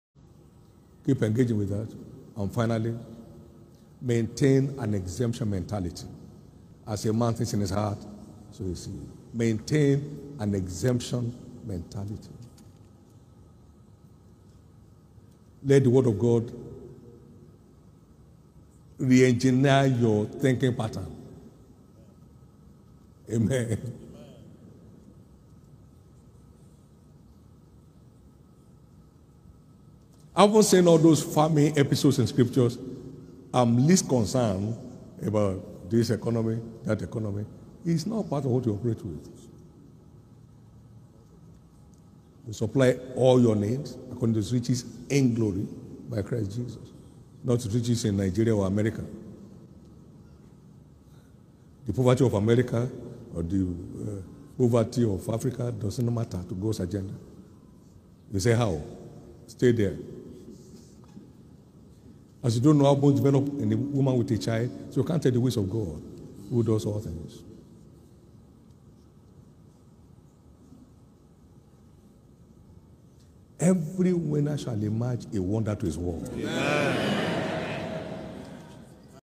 THE REASON I AM NOT BOTHERED ABOUT THE ECONOMY OF ANY NATION - Bishop David Oyedepo
GOD WILL SUPPLY ALL YOUR NEEDS ACCORDING TO HIS RICHES IN GLORY BY CHRIST JESUS, NOT HIS RICHES IN NIGERIA OR AMERICA.